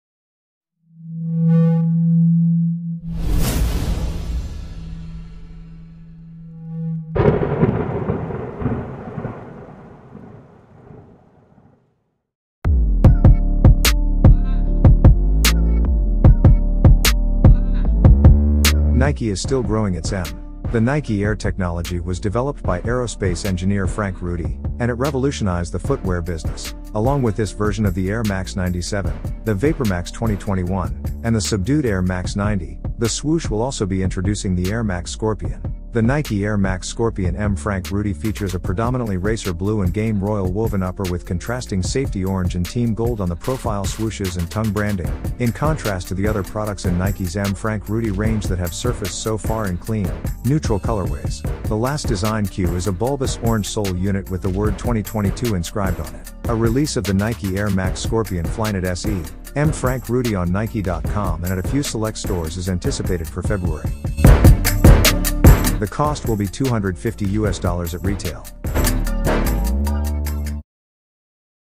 M. FRANK RUDY RACER BLUE 2023 Nike Air Max Scorpion Flyknit SE DETAILED LOOK + PRICE
NIKE AIR MAX SCORPION FLYKNIT SE
M. FRANK RUDY
RACER BLUE
2023

FOR ADULTS
LIFESTYLE

STYLE CODE: DX4768-400
COLORS: RACER BLUE - SAFETY ORANGE

SRP: US$250
RELEASE DATE: FEBRUARY 2023 TBA